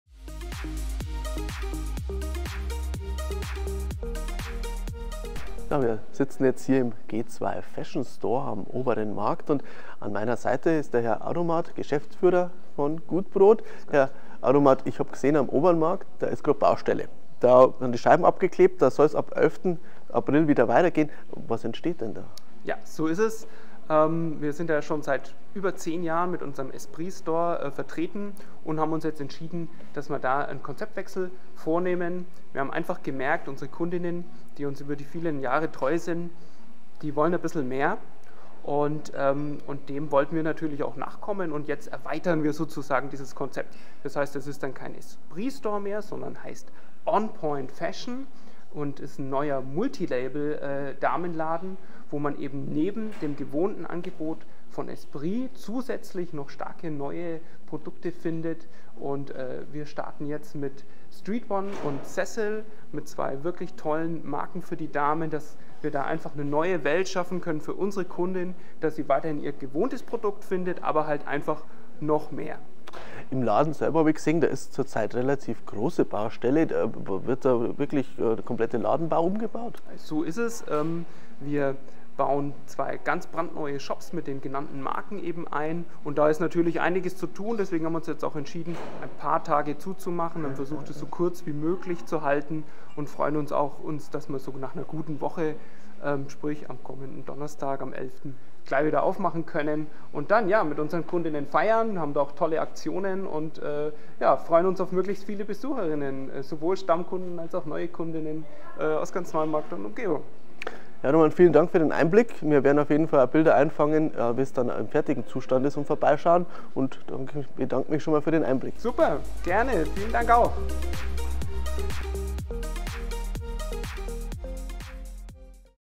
On Point Fashion eröffnet am 11.4. in der Oberen Marktstraße
Seit über zehn Jahren begrüßte der Esprit Store in der Oberen Marktstraße 37 in Neumarkt die Kunden. Nun fand dort ein Konzeptwechsel statt und der Laden verwandelt sich in "On Point Fashion".

Mit dem Geschäftsführer des Unternehmens Gutbrod, Alexander Adomat, haben wir uns unterhalten was dort genau passiert und welche zusätzlichen Marken dort einziehen.

On Point Fashion
Obere Marktstraße 37, 92318 Neumarkt
Mo-Fr 10-18 Uhr / Sa. 10-16 Uhr